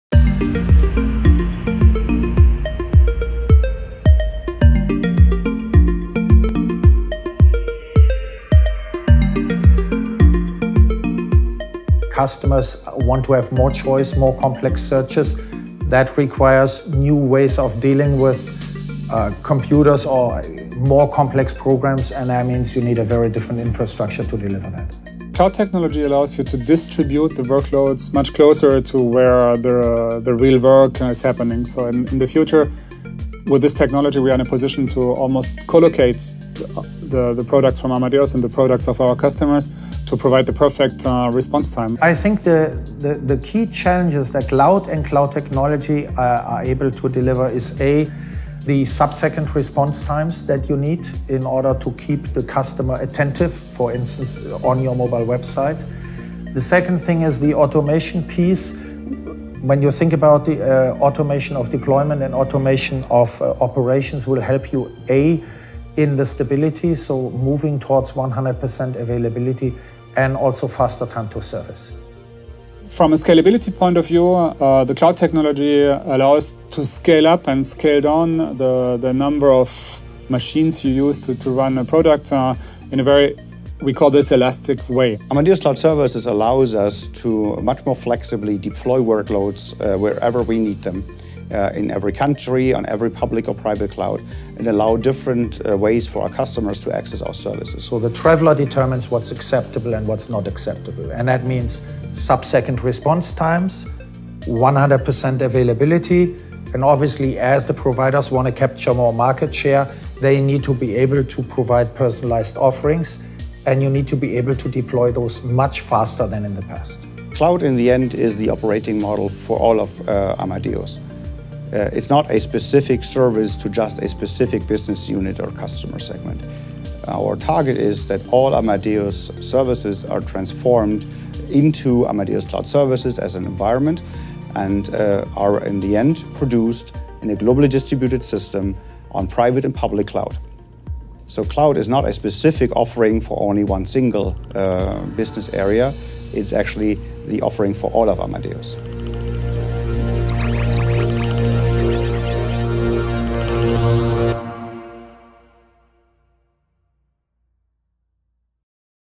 Amadeus – advancing travel technology with cloud
Amadeus is taking advantage of cloud-based technology to improve performance, agility and flexibility for our customers. For more stories visit Amadeus.com/technology